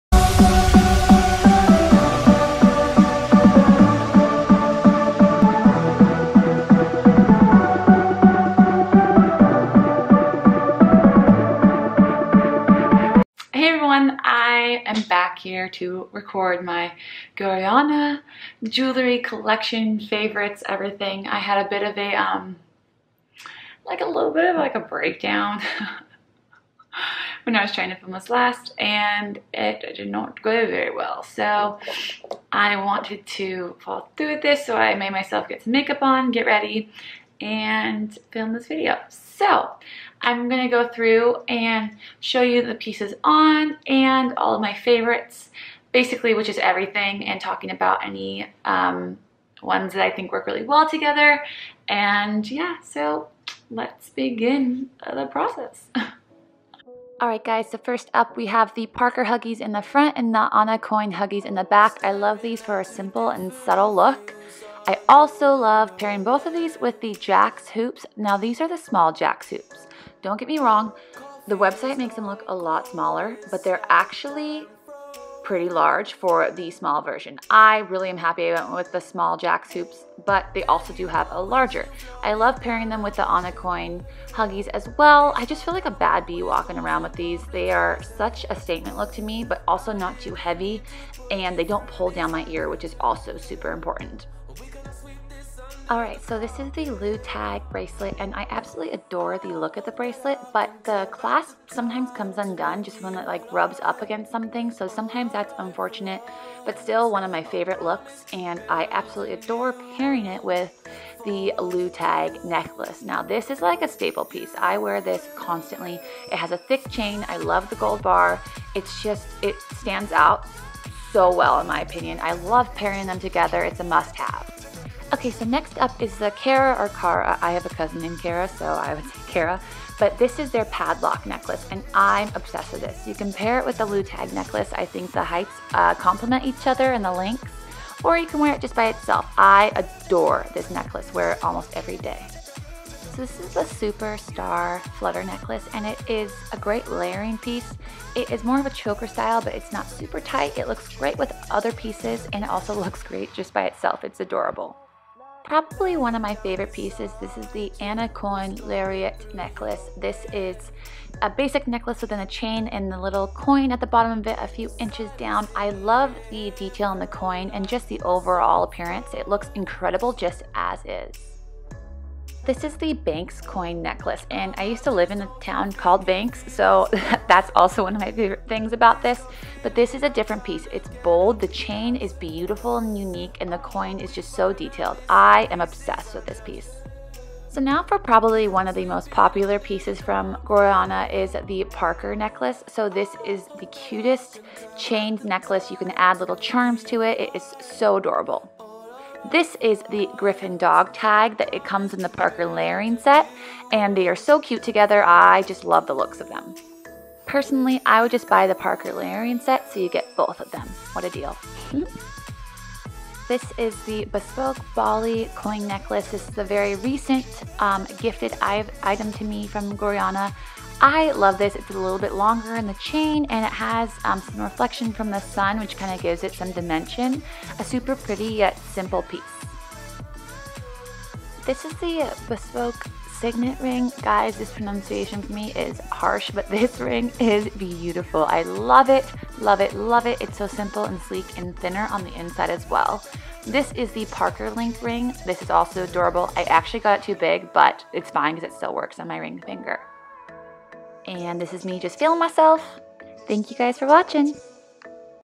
Jewelry Review: Gorjana Favorites!
One of my favorite jewelry brands is Gorjana and I couldn't wait to share with you guys a break down of my favorite pieces! Hope you enjoy :)